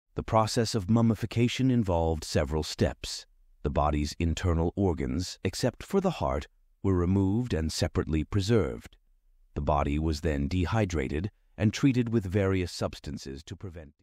Mummification Process
Certainly! Here are some interesting facts about mummification in ancient Egypt:

1. **Purpose of Mummification:** Ancient Egyptians believed in an afterlife, and the process of mummification was developed to preserve the body for the journey to the afterlife.

2. **Mummification Process:** The process of mummification involved several steps. The body's internal organs, except for the heart, were removed and separately preserved. The body was then dehydrated and treated with various substances to prevent decay.

3. **Canopic Jars:** The preserved organs were stored in containers known as canopic jars. Each jar was associated with a specific organ and protected by a deity. The four sons of Horus—Imsety, Hapy, Duamutef, and Qebehsenuef—were often represented on the lids of these jars.

4. **Role of Anubis:** Anubis, the god of embalming and mummification, played a significant role in the mummification process. It was believed that Anubis guided the deceased through the afterlife.

5. **Natron:** Natron, a naturally occurring mixture of sodium carbonate and sodium bicarbonate, was a key element in the mummification process. It helped in the dehydration of the body and acted as a preservative.

6. **Wrapping the Body:** After the body was prepared, it was wrapped in linen bandages. Amulets and charms were often placed among the bandages to protect the deceased in the afterlife.

7. **Book of the Dead:** The Book of the Dead, a collection of spells and illustrations, was often buried with the deceased to guide them through the afterlife and ensure a safe journey.

8. **Social Status and Mummification:** Mummification was initially reserved for pharaohs and elite individuals due to its cost. However, over time, even individuals of lower social status began to receive some form of mummification.

9. **Animal Mummies:** It wasn't just humans who were mummified. Animals, especially those associated with certain deities like cats, dogs, and ibises, were also mummified and buried as offerings to the gods.

10. **King Tutankhamun's Mummy:** The mummy of King Tutankhamun, discovered in 1922, is one of the most well-known mummies in the world. His burial chamber contained a gold mask, jewelry, and other valuable items.

11. **Mummification Decline:** As Egypt underwent cultural and religious changes, mummification practices declined. The rise of Christianity in Egypt contributed to the decline of traditional burial practices.

12. **Modern Study of Mummies:** Modern technology, such as CT scans and DNA analysis, has allowed scientists to study mummies without unwrapping them, providing insights into ancient health, diet, and diseases.

Mummification is a fascinating aspect of ancient Egyptian culture and religion, reflecting their beliefs in the afterlife and the importance of preserving the body for the journey beyond death.